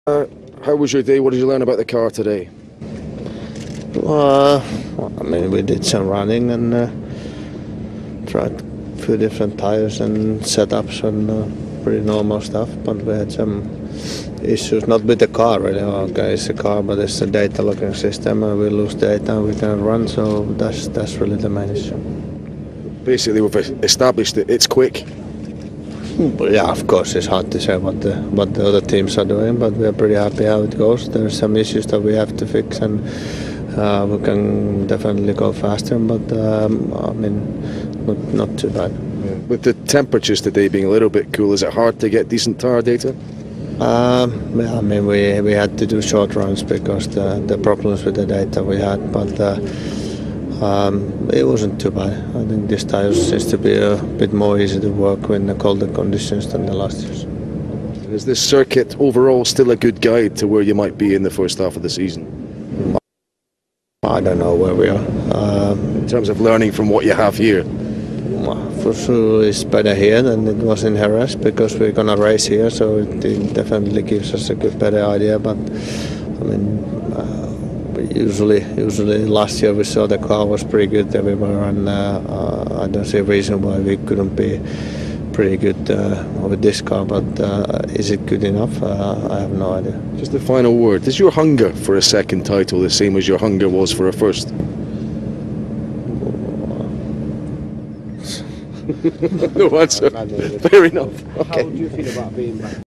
Kimi Räikkönen interview - Refuses to answer stupid question
Funny interview with Kimi Räikkönen at the Barcelona pre-season tests 2013. As usual Kimi is laid back and does not seem interested in answering questions and even refuses to answer the reporters stupid question in the end :D